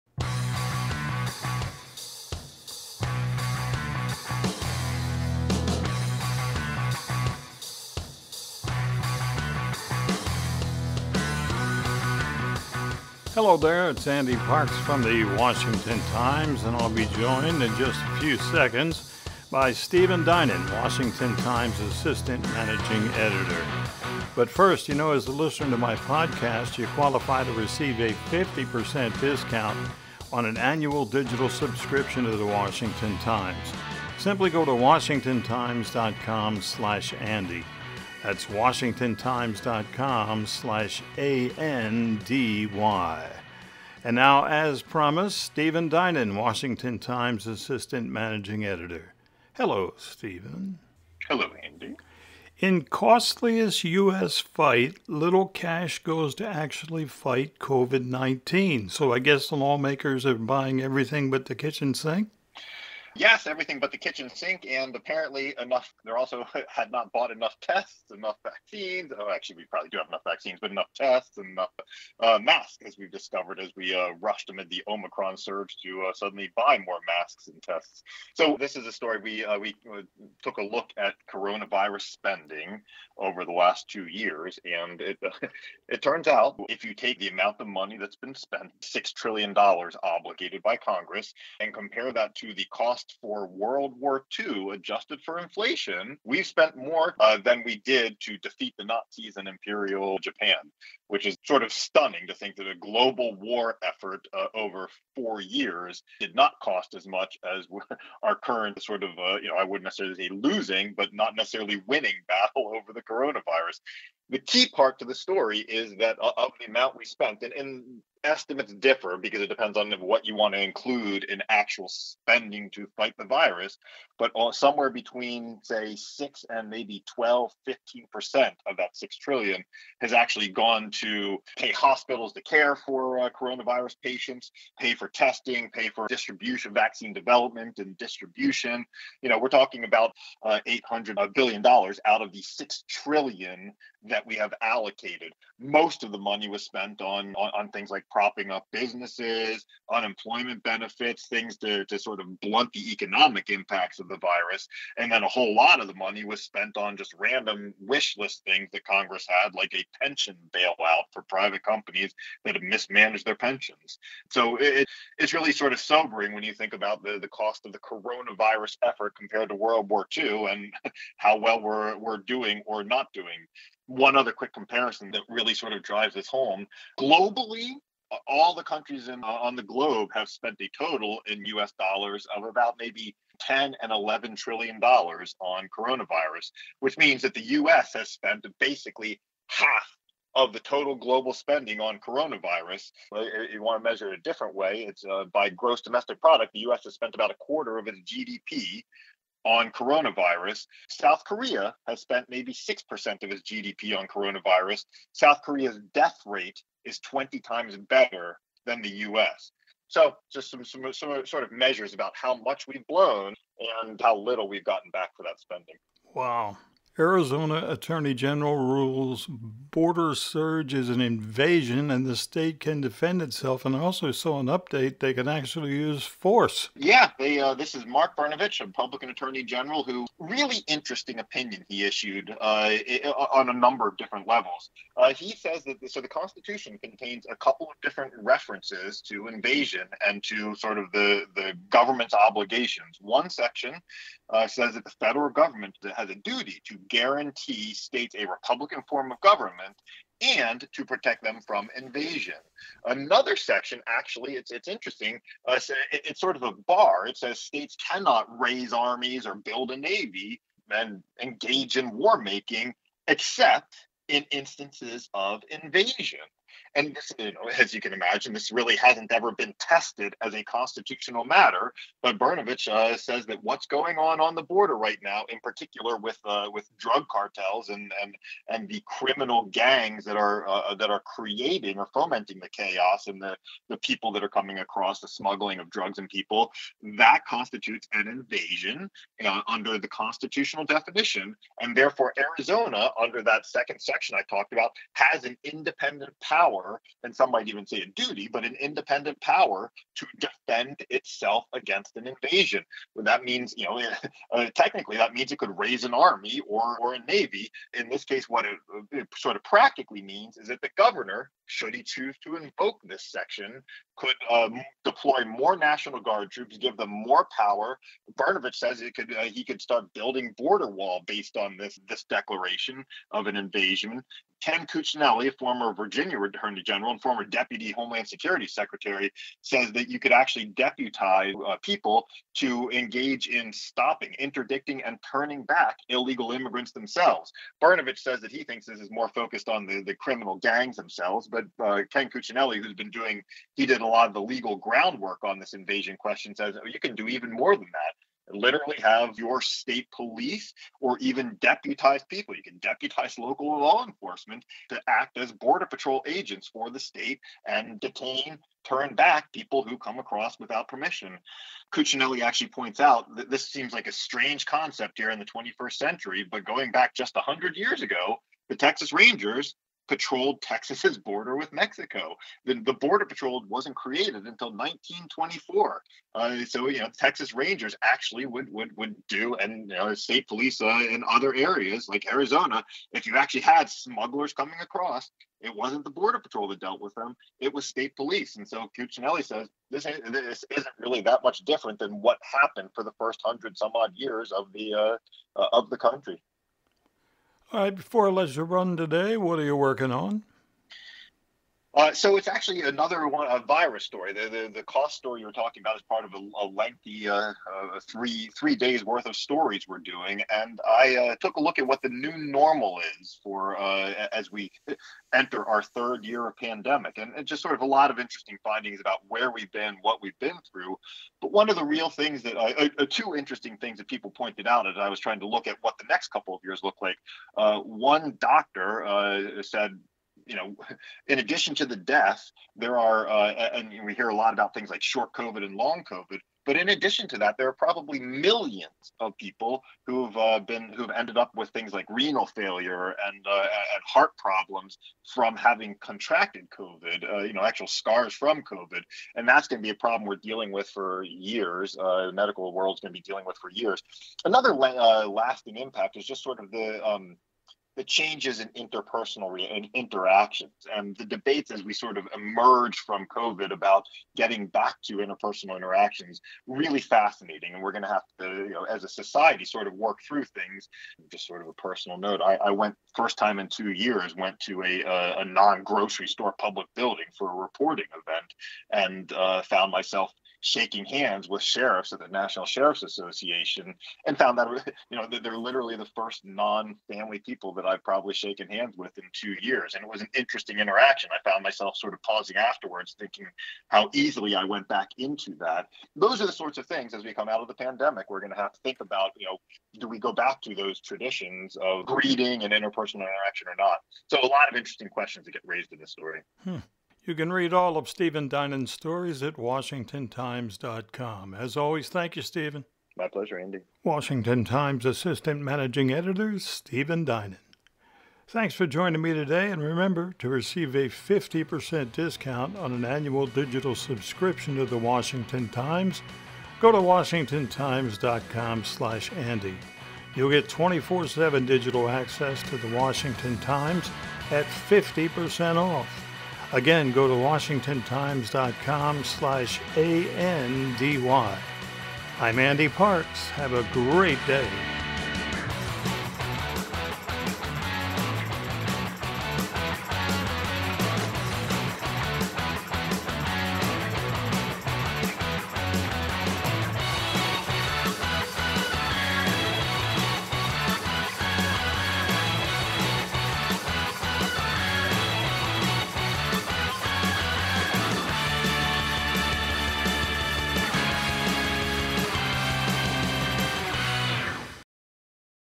Stephen Dinan: Blowing money for little return, COVID cash not going to fight virus
Up first, Stephen Dinan tells Andy about the first in a six-part series on the two years of coronavirus spending. Uncle Sam has spent only a small fraction of that money — 15% or less, depending on who’s counting — on beating the coronavirus itself. Then, as Stephen reports, Arizona Attorney General Mark Brnovich issued an official opinion saying the surge of illegal immigrants coming across the border meets the legal definition of an “invasion” under the U.S. Constitution. Brnovich‘s opinion is the latest salvo in a war of rhetoric over how bad the border has gotten under President Biden.